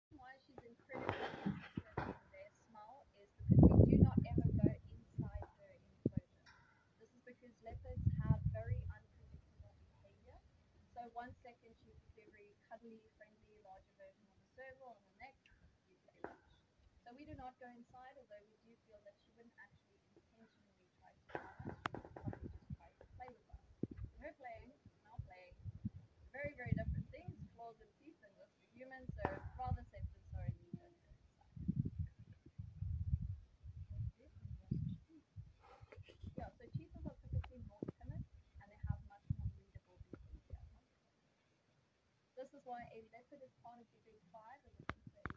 Leopard Musina demonstrating the Flehman Grimace
Have you heard of the Flehman Grimace or Flehman Response? This is a behaviour often demonstrated by many mammals (excluding humans) where they can be seen opening their mouths and “grinning” to take in chemical signals (pheromones) left behind by other animals. This ability is possible due to what is known as the Jacobson’s Organ, which is connected to the nose and mouth. Here Musina is demonstrating the Flehman Grimace and can be seen inspecting the water bowl. What is interesting to note, is that this water bowl was recently cleaned and left there by HESC curator Anya and guide Antoinette.